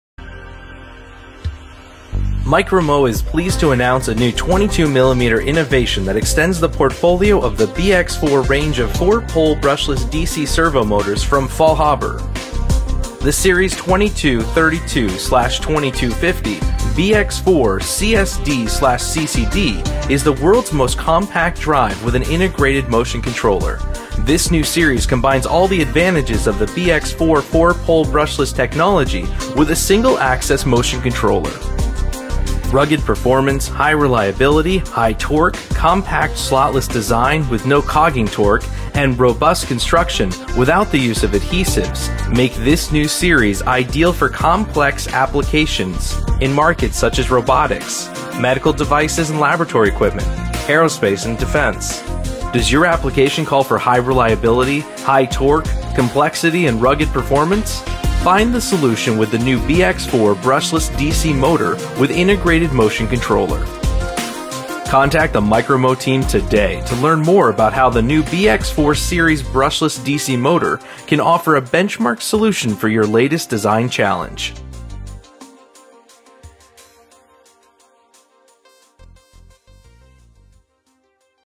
MICROMO Presents the FAULHABER BX4 2232/2250 Brushless DC Motors
The series 2232 / 2250...BX4 CSD/CCD from Faulhaber combines all the advantages of the BX4 four pole brushless technology with a single axis motion control.
Key benefits of the new BX4 :
o High reliability
o High torque
o Rugged performance

Key application fits include robotics, medical devices and aerospace and defense.